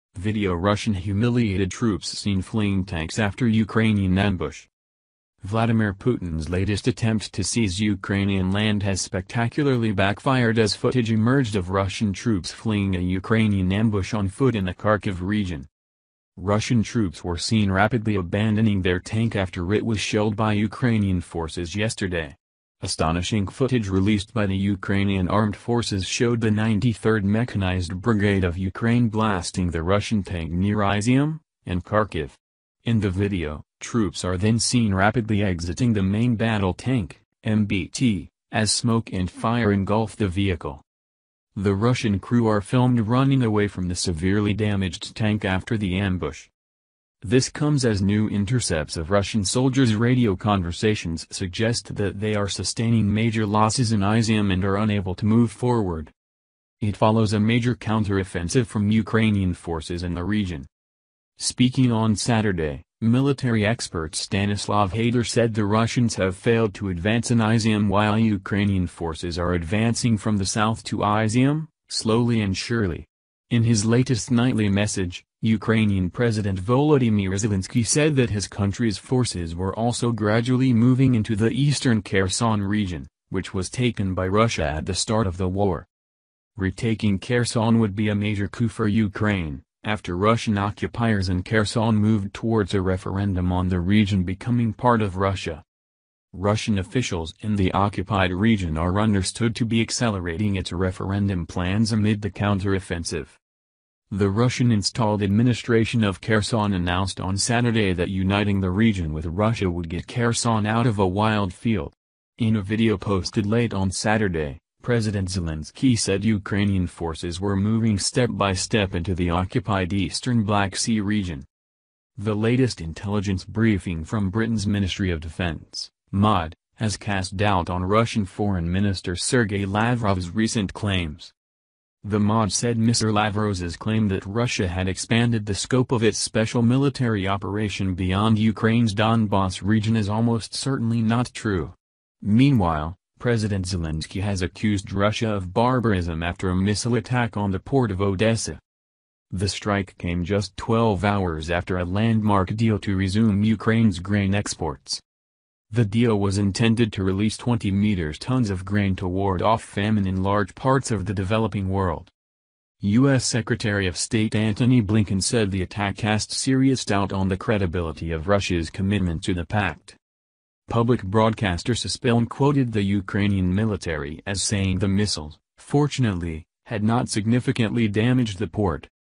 PUTIN Humiliated As Video Russian Troops Seen Fleeing Tanks After Ukrainian Ambush
Video Russian humiliated troops seen fleeing tanks after Ukrainian ambush
VLADIMIR PUTIN'S latest attempt to seize Ukrainian land has spectacularly backfired as footage emerged of Russian troops fleeing a Ukrainian ambush on foot in the Kharkiv region.
Russian troops were seen rapidly abandoning their tank after it was shelled by Ukrainian forces yesterday. Astonishing footage released by the Ukrainian Armed Forces showed the 93rd Mechanized Brigade of Ukraine blasting the Russian tank near Izium, in Kharkiv. In the video, troops are then seen rapidly exiting the main battle tank (MBT) as smoke and fire engulf the vehicle.

The Russian crew are filmed running away from the "severely damaged tank" after the ambush.